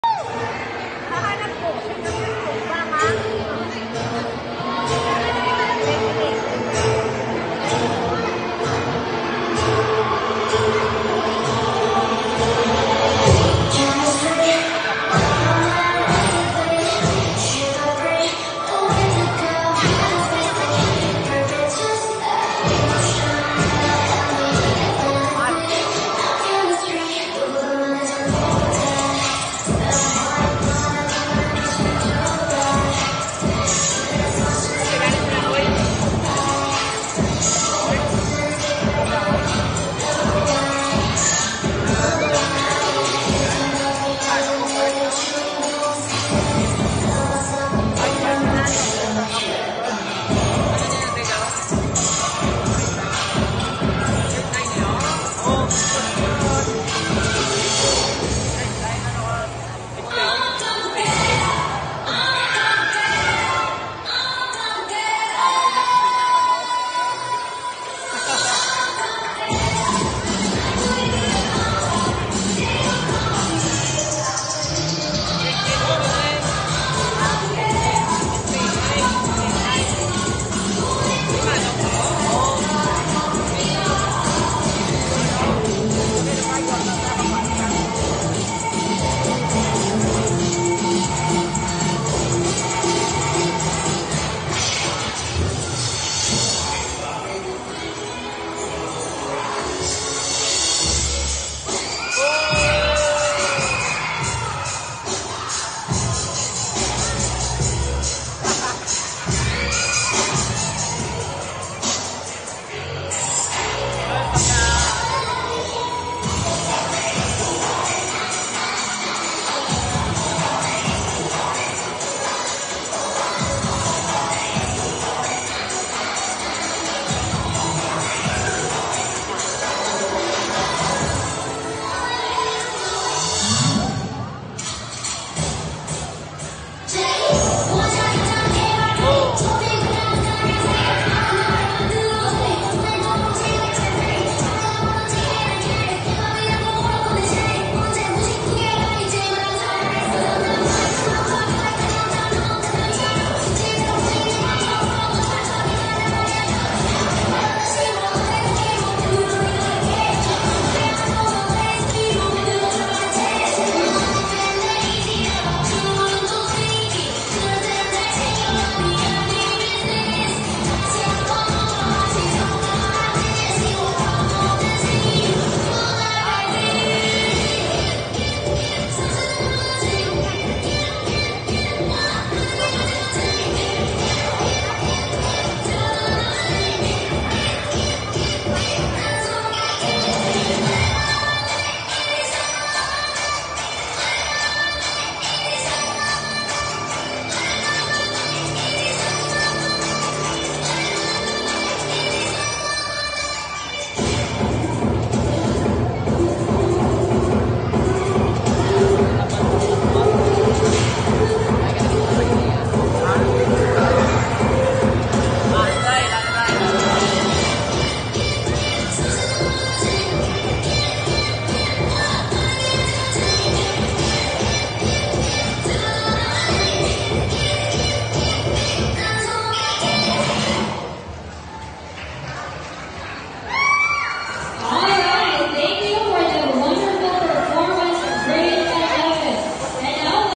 PopDance Grade 11 (PURPLE VIPERS) in DMLMHS | Julian Baylon
I havent been uploading something since last last year, because of being busy in school. But here is one of my dance experience again in Grade 11. I might be uploading some videos soon.